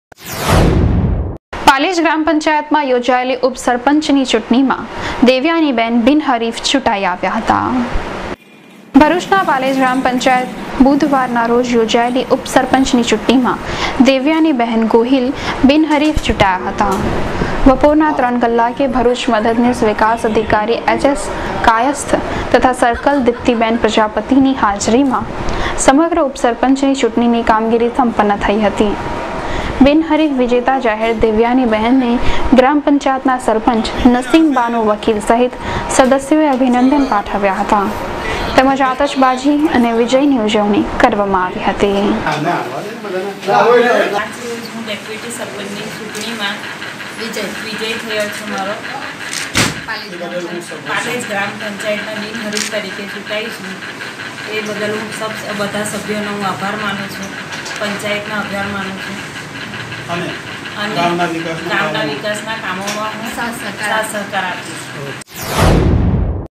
DNTV NEWS CHANNEL :- 09-10-2019 ( પાલેજ )
પાલેજ ગ્રામ પંચાયતની યોજાયેલી ઉપસરપંચની ચુંટણીમાં દેવયાનીબેન બિનહરીફ ચૂંટાયેલા જાહેર થયા હતા